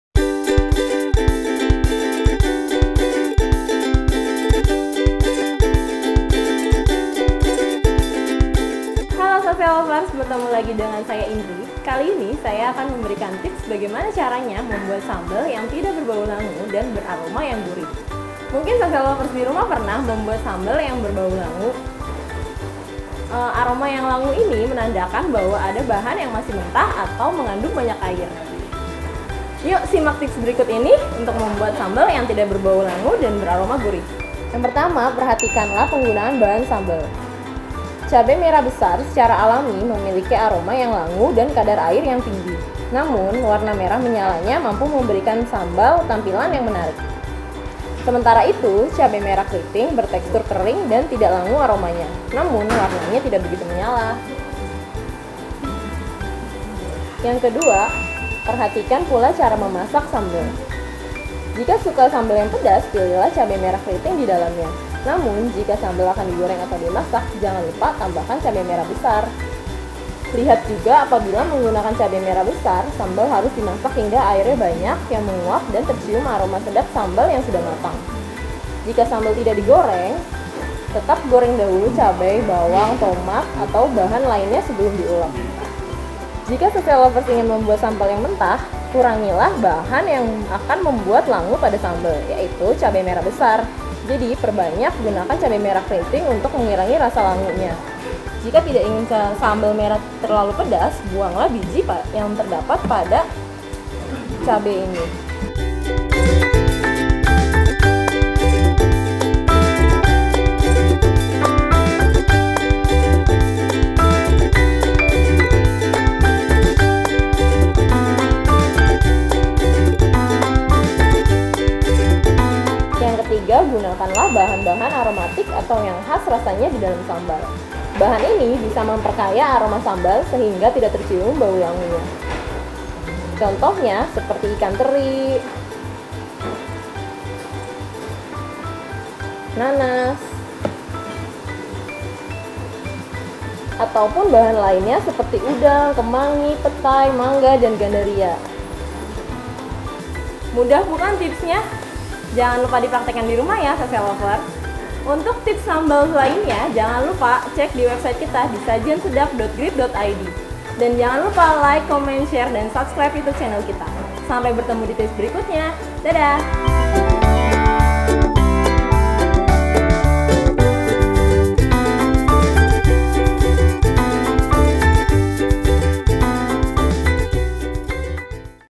Ini Dia Cara Membuat Sambal Agar Tidak Berbau Langu
Membuat Sambal Agar Tidak Berbau Langu

Sambal enggak akan berbau langu kalau tips ini sudah kita praktikan di rumah!

Produser:
Penulis Naskah:
Peraga:
Fotografer: Inggar Rivaldi
Videografer: Bambang Tetuko dan Inggar Rivaldi
Video Editor: Bambang Tetuko